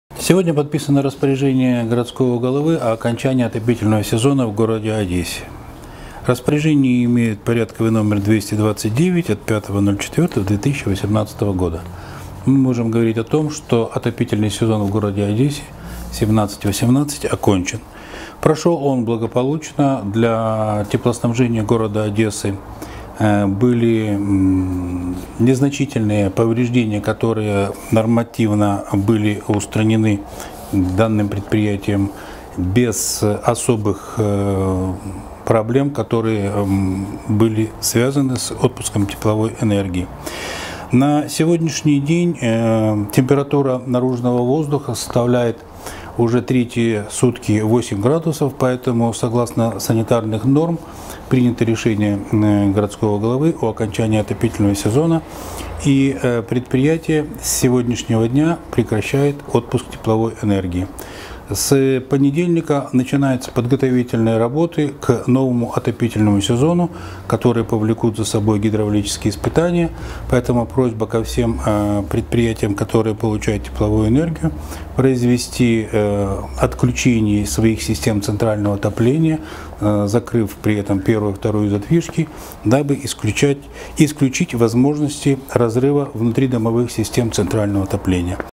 В Одессе завершается отопительный сезон
6 апреля 2018 года, в Одессе завершается отопительный сезон. Данное решение принято с учетом строительных норм и правил.

Соответствующее распоряжение «Об окончании отопительного сезона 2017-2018 гг.» подписано  5 апреля 2018 года.

Руководителям предприятий всех форм собственности, на балансе которых находятся системы центрального отопления, поручено обеспечить отключение данных систем.

Коммунальному предприятию «Теплоснабжение города Одессы», руководителям предприятий, на балансе которых находятся системы центрального отопления и системы горячего водоснабжения, поручено начать выполнение планового ремонта теплотрасс и систем центрального отопления согласно утвержденному графику.

Комментарий заместителя директора департамента городского хозяйства Сергея Агуца